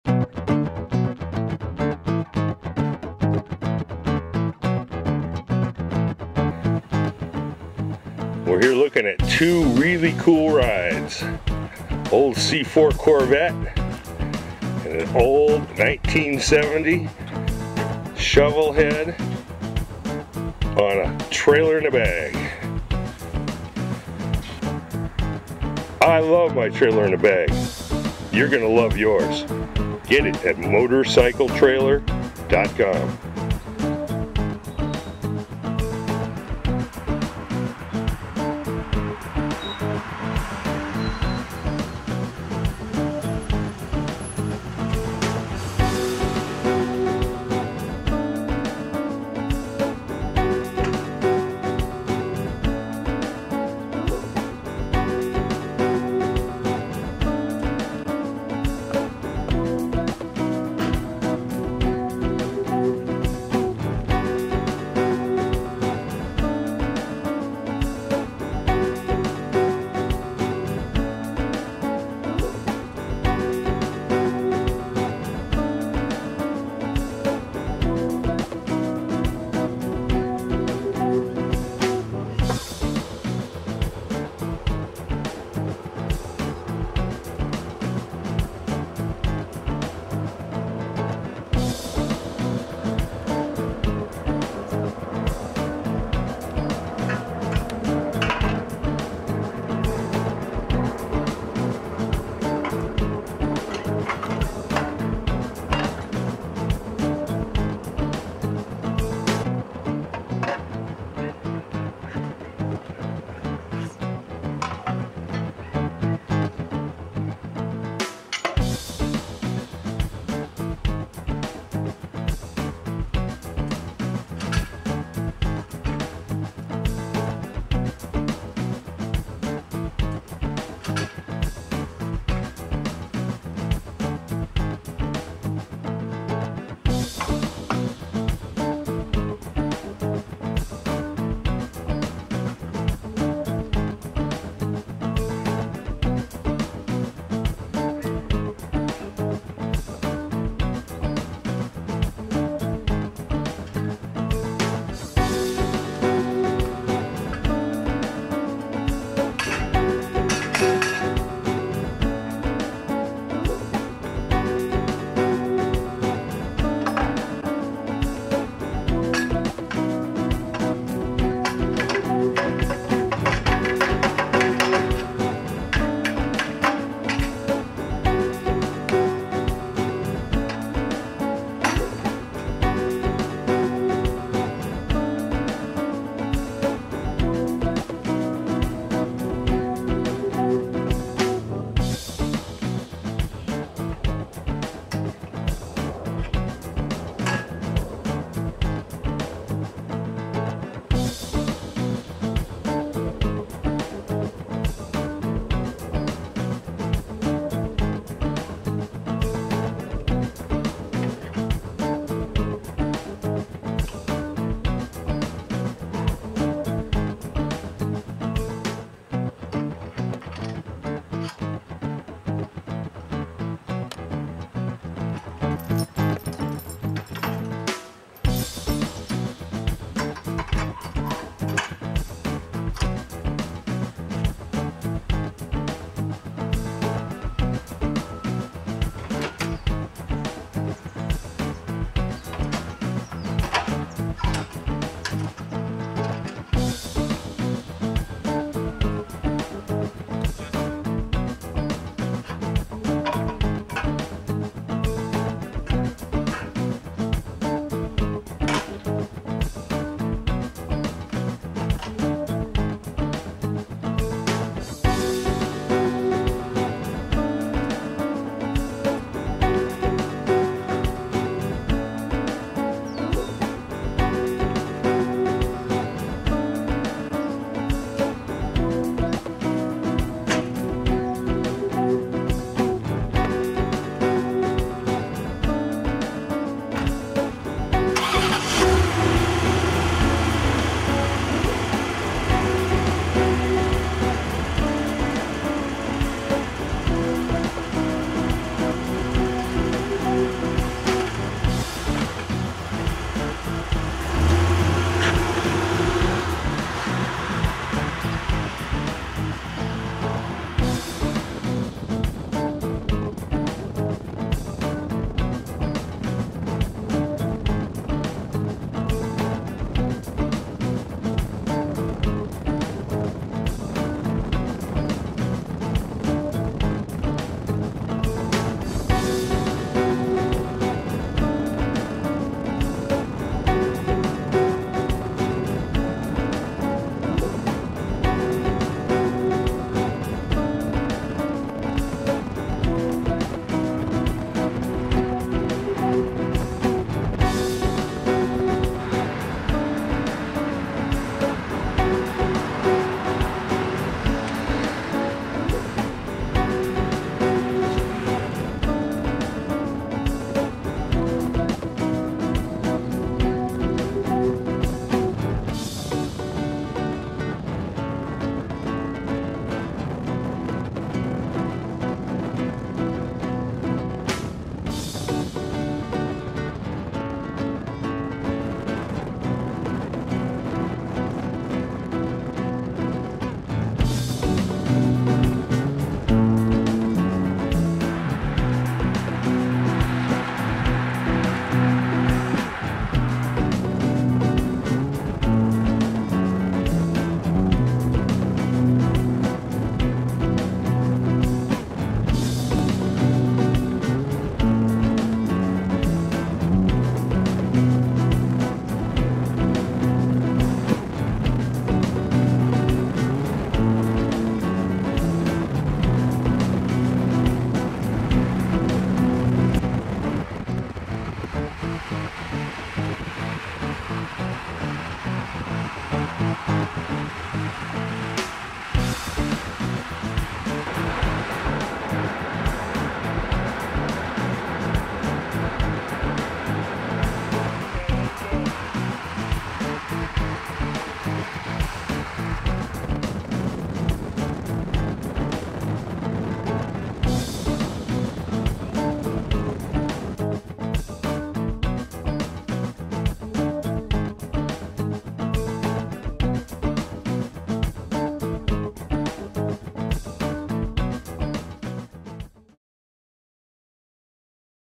7min Trailer in A Bag Assembed Loading@HIWAY Speeds behind a Corvette
Update Dec 2016  Trailer in a Bag is back in production.  Call for details!!  Larry at motorcycle trailer Look here at  Corvette towing the Harley shovel head get it on the Trailer in a Bag.